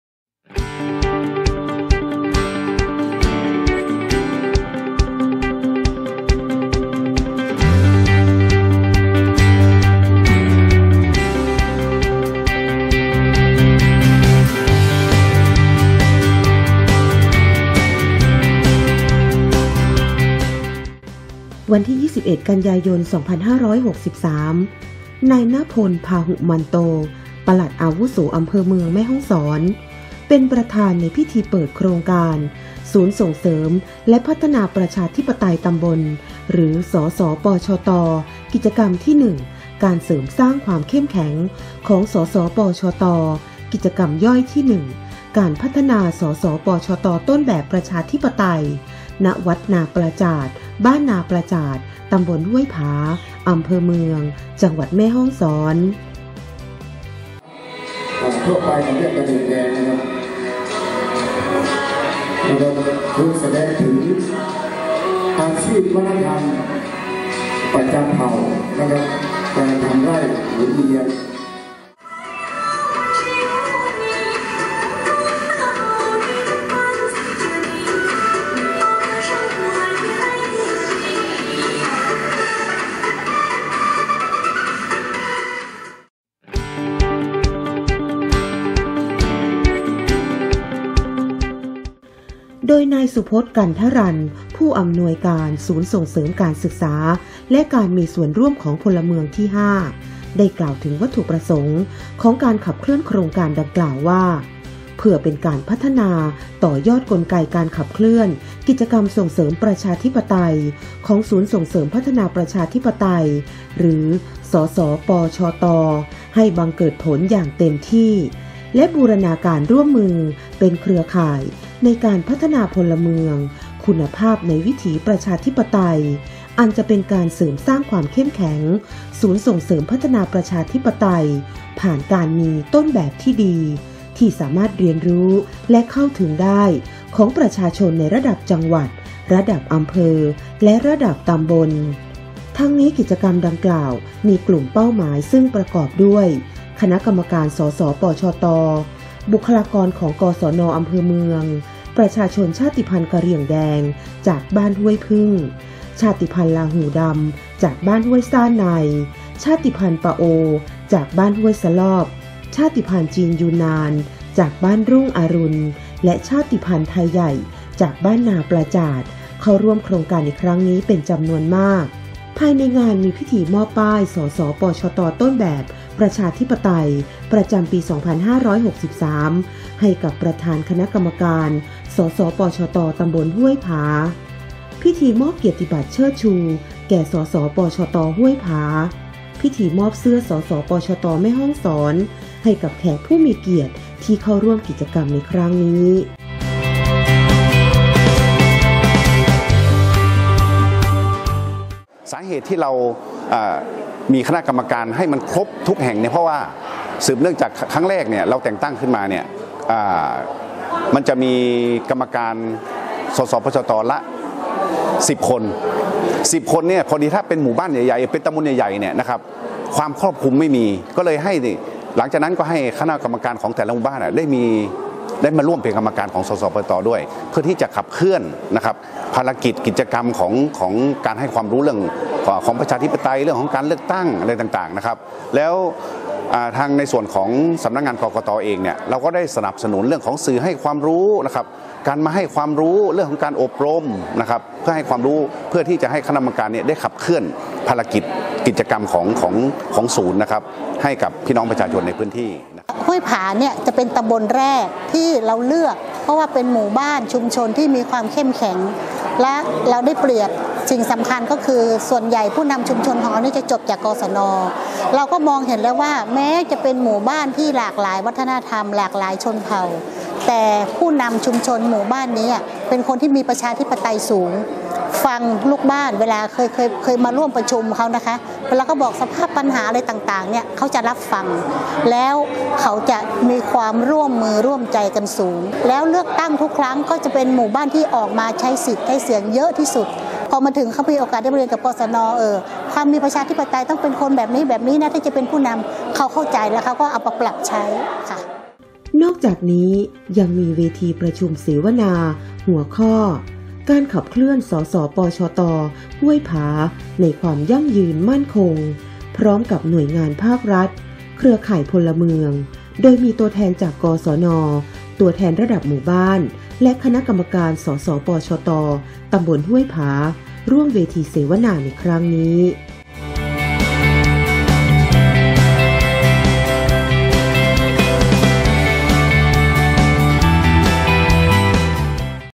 กกต จังหวัดแม่ฮ่องสอน ขับเคลื่อนโครงการศูนย์ส่งเสริมและพัฒนาประชาธิปไตยตำบล หรือ ศส ปชต ตำบลห้วยผา
กกต.จังหวัดแม่ฮ่องสอน ขับเคลื่อนโครงการศูนย์ส่งเสริมและพัฒนาประชาธิปไตยตำบล หรือ ( ศส.ปชต.) ตำบลห้วยผา เน้นสร้างการรับรู้แก่พี่น้องชาติพันธุ์ต่างๆ ในพื้นที่
วันที่ 21 กันยายน 2563 นายณพล พาหุมันโต ปลัดอาวุโสอำเภอเมือง เป็นประธานในพิธีเปิดโครงการศูนย์ส่งเสริมและพัฒนาประชาธิปไตยตำบล หรือ ( ศส.ปชต.) กิจกรรมที่ 1 การเสริมสร้างความเข้มแข็งของ ศส.ปชต.  กิจกรรมย่อยที่ 1 การพัฒนา ศส.ปชต.ต้นแบบประชาธิปไตย  ณ วัดนาปลาจาด บ้านนาปลาจาด ต.ห้วยผา อ.เมือง จ.แม่ฮ่องสอน
นายสุพจน์ กันทรัญ ผู้อำนวยการศูนย์ส่งเสริมการศึกษาและการมีส่วนร่วมของพลเมืองที่ 5 ( ศสร.5) ได้กล่าวถึงวัตถุประสงค์ของการขับเคลื่อนโครงการดังกล่าวว่า  เพื่อเป็นการพัฒนาต่อยอดกลไกการขับเคลื่อนกิจกรรมส่งเสริมประชาธิปไตย ของศูนย์ส่งเสริมพัฒนาประชาธิปไตย หรือ ( ศส.ปชต.) ให้บังเกิดผลอย่างเต็มที่ และบูรณาการร่วมมือเป็นเครือข่ายในการพัฒนาพลเมือง คุณภาพในวิถีประชาธิปไตย อันจะเป็นการเสริมสร้างความเข้มแข็งศูนย์ส่งเสริมพัฒนาประชาธิปไตย ผ่านการมี “ ต้นแบบที่ดี “ ที่สามารถเรียนรู้และเข้าถึงได้ ของประชาชนในระดับจังหวัด ระดับอำเภอ และระดับตำบล
ทั้งนี้ กิจกรรมดังกล่าวมีกลุ่มเป้าหมาย ประกอบด้วย คณะกรรมการ ศส.ปชต. บุคลากรของ กศน.อำเภอเมือง ประชาชนชาติพันธุ์กะเหรี่ยงแดงจากบ้านห้วยผึ้ง ชาติพันธุ์ลาหู่ดำ จากบ้านห้วยส้านใน  ชาติพันธุ์ปะโอ จากบ้านห้วยซลอบ ชาติพันธุ์จีนยูนาน จากบ้านรุ่งอรุณ และชาติพันธุ์ไทใหญ่ จากบ้านนาปลาจาด เข้าร่วมโครงการในครั้งนี้เป็นจำนวนมาก
ภายในงานมีพิธีมอบป้าย ศส.ปชต.ต้นแบบประชาธิปไตย ประจำปี 2563 ให้กับประธานคณะกรรมการ ศส.ปชต.ตำบลห้วยผา พิธีมอบเกียรติบัตรเชิดชู แก่ ศส.ปชต.ห้วยผา พิธีมอบเสื้อ ศส.ปชต.แม่ฮ่องสอน ให้กับแขกผู้มีเกียรติที่เข้าร่วมกิจกรรม
นอกจากนี้ ยังมีเวทีประชุมเสวนา หัวข้อ “ การขับเคลื่อน ศส.ปชต. ห้วยผา ในความยั่งยืน มั่นคง “ พร้อมกับหน่วยงานภาครัฐ เครือข่ายพลเมือง โดยมีตัวแทนจาก กศน. ตัวแทนระดับหมู่บ้าน และคณะกรรมการ ศส.ปชต.ตำบลห้วยผา ร่วมเวทีเสวนาในครั้งนี้.